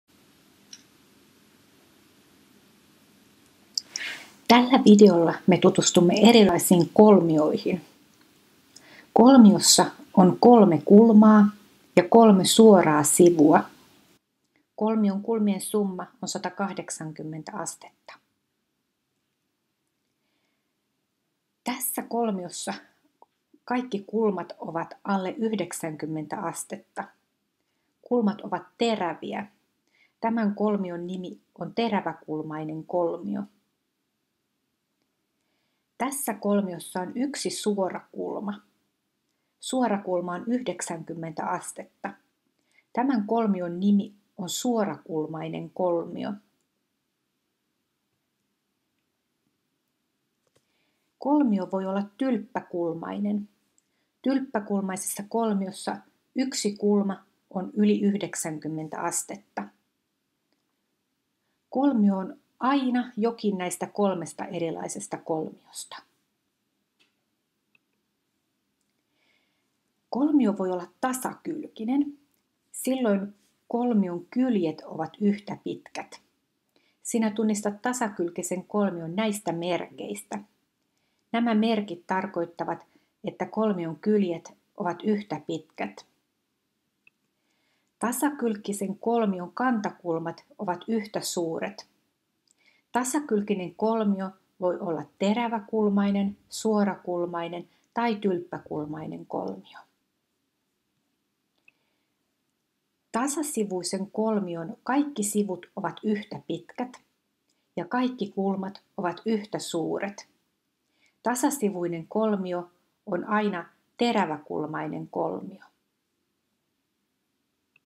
Erilaiset kolmiot
Tällä videolla sinä opit tunnistamaan ja nimeämään erilaiset kolmiot.

Video on tehty erityisesti opiskelijoille, jotka opiskelevat suomea toisena kielenä.